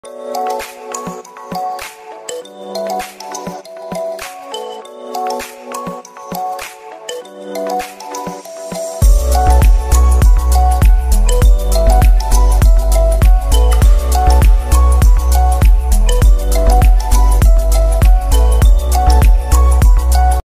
Opening Day 2 Of The Mini Crystal Calendar!
Stqy tuned for Day 3! And have a very merry Christmas! Also the reason why I uploaded Day 1 and Day 2 today is because yesterday the Day 1 video said unable to preview! So I'm sorry for the errors!